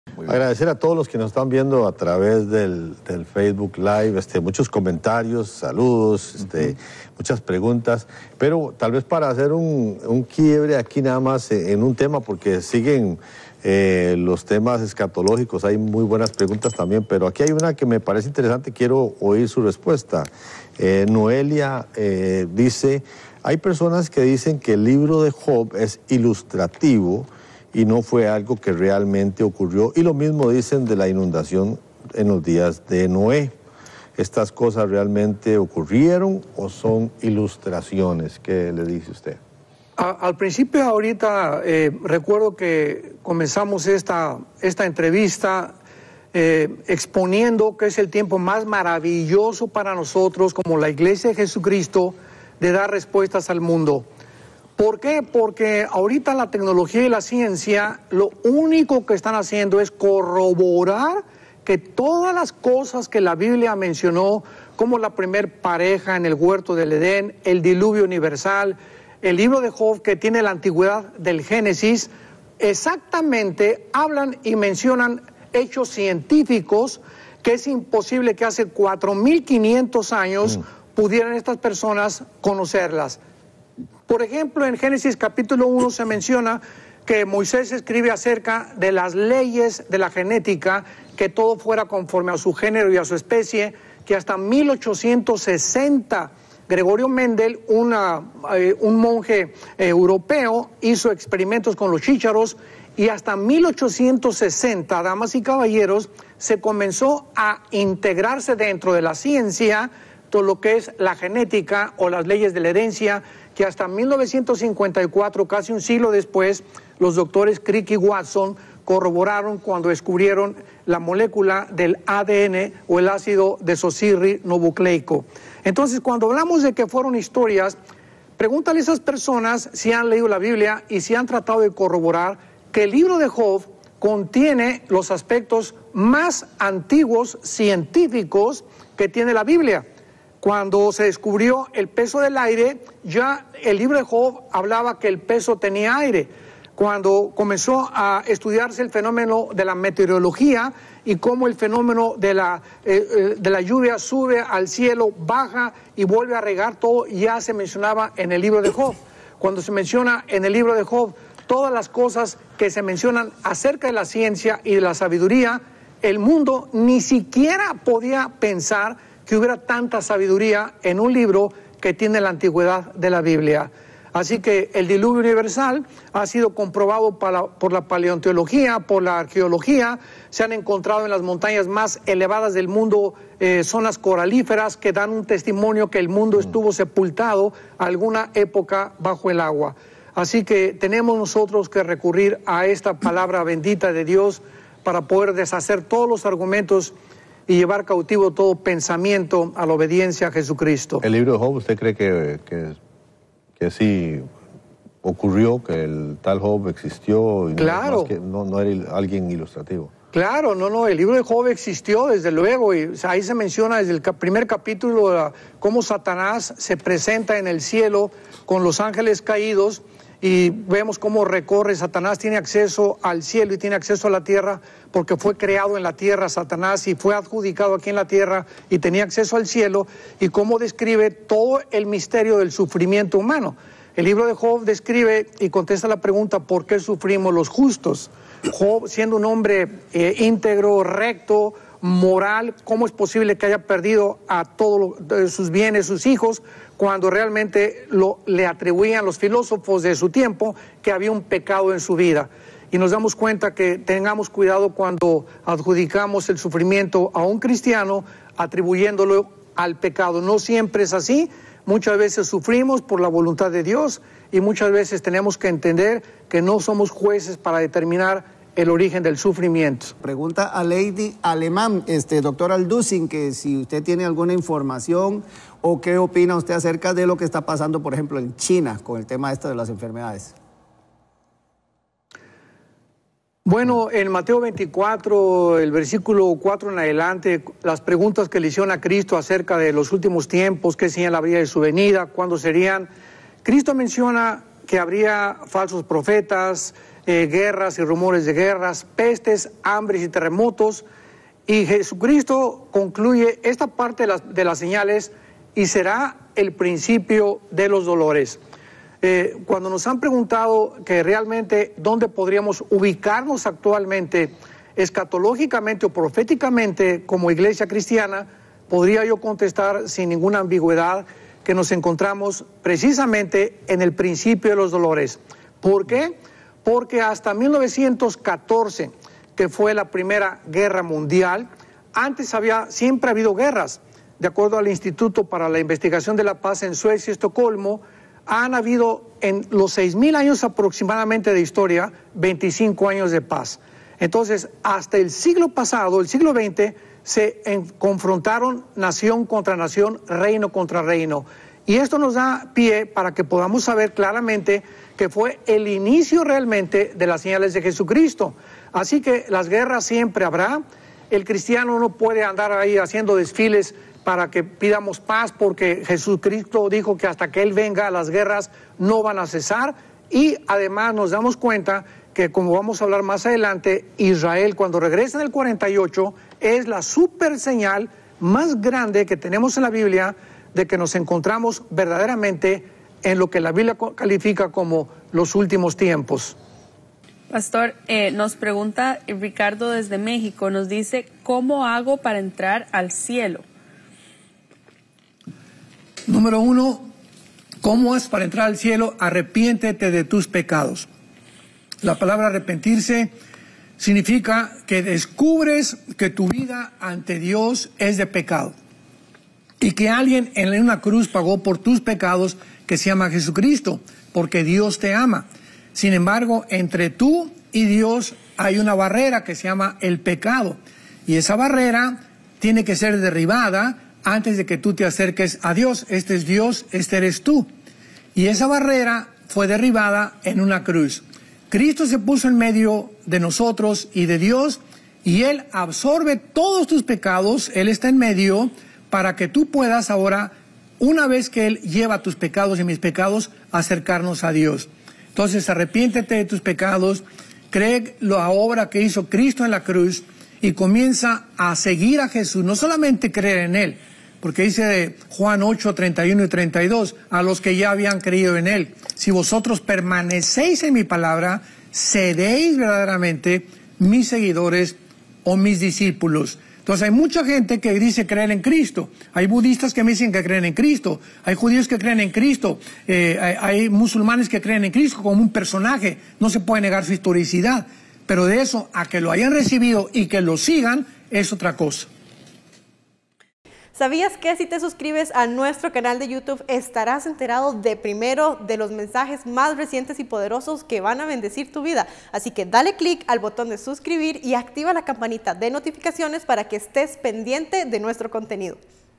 ¿El diluvio, Job, etc. son solo ilustraciones en la Biblia? - Armando Alducin responde - Enlace TV
En este video, el pastor Armando Alducin responde a la pregunta:
hay personas que el libro de Job es ilustrativo y no es algo que realmente ocurrió, también lo dicen de la inundación en los días de Noé ¿estas cosas realmente ocurrieron?
También llega la pregunta: respecto a lo que está pasando en China con las enfermedades ¿estamos ya en el principio de dolores?
También desde México preguntan. ¿cómo le hago para entrar al cielo?
Escucha la respuesta del pastor Alducin